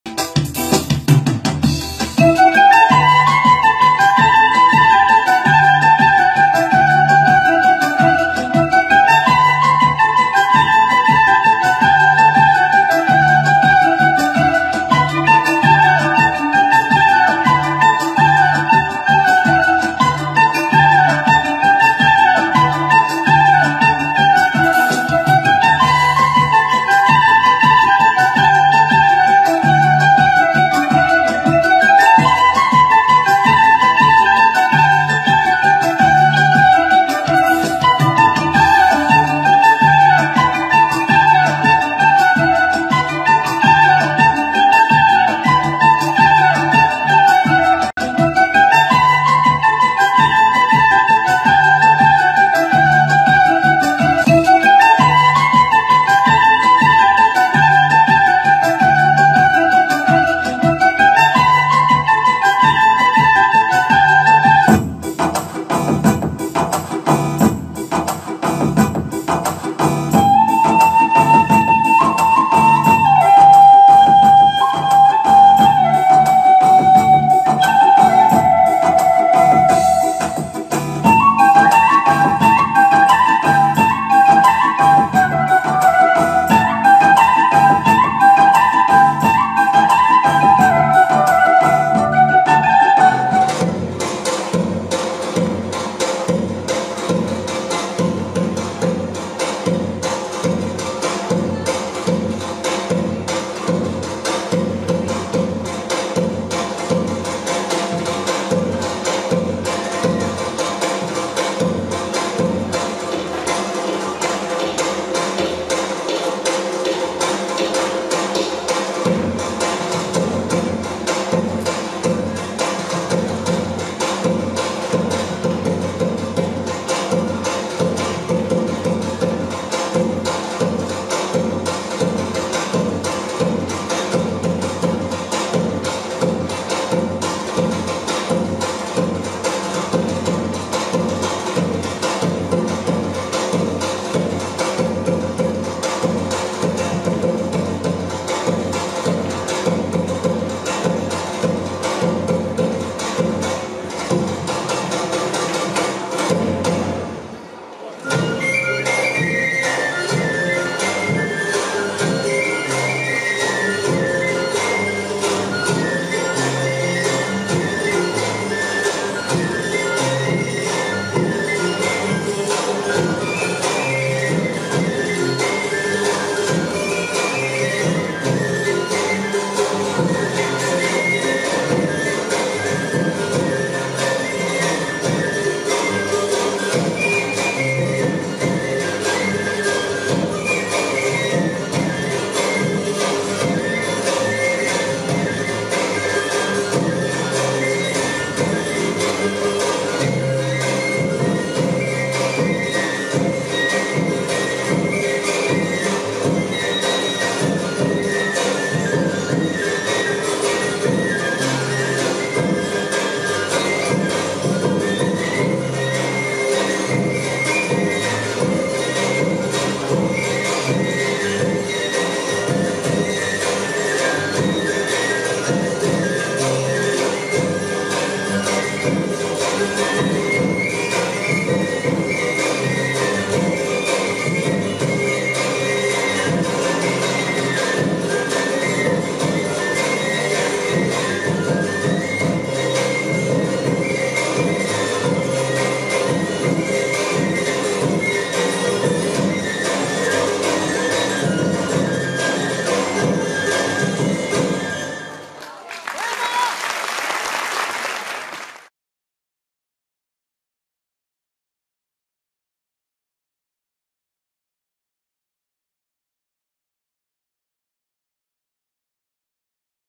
Bukuri Artistike Mirditore
Bukuri Artistike Mirditore . Me vajzat e grupit te valleve Rreshen